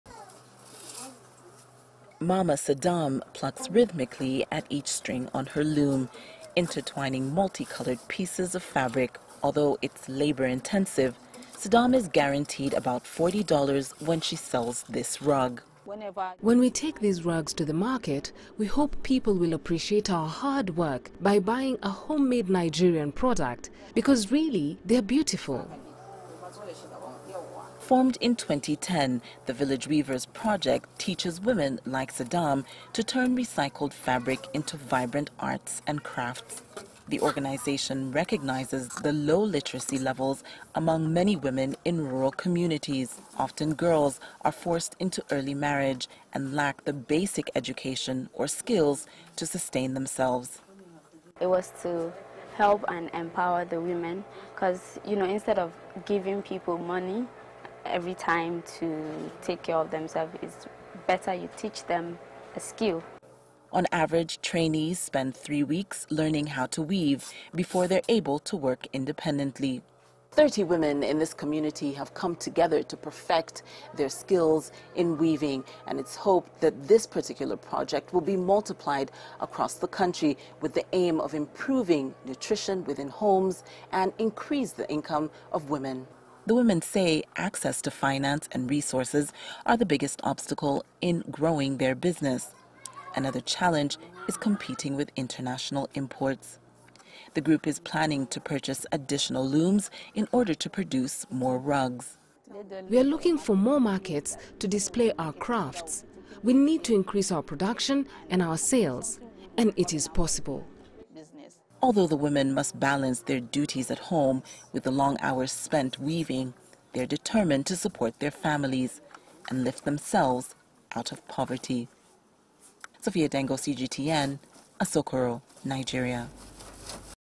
Weaving skills economically empower women in Nigeria
In Nigeria, a non-profit organization is aiming to empower women in some of the country's poorest areas. It's offering training courses in weaving to give women skills that can help them earn a living. The UN estimates over 40-percent of the country's female population is illiterate - which makes it difficult for them to engage in economic activities. Sophia Adengo reports from Abuja.